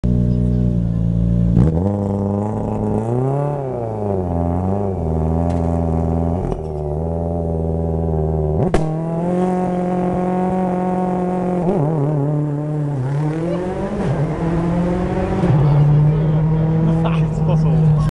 Ford Focus WRC Launch Control Turán Frigyes
BP Rally . Ford Focus WRC launch controll start at BudaPest Rally .
Great Sound !!!